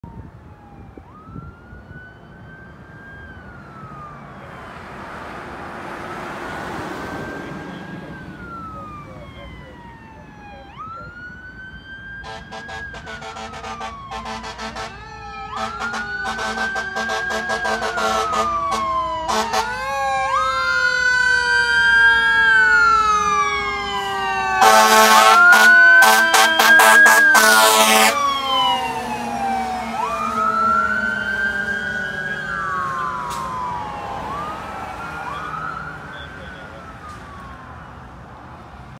Chanhassen Engine 1 Responding
Chanhassen Engine 1 responding to a PI crash.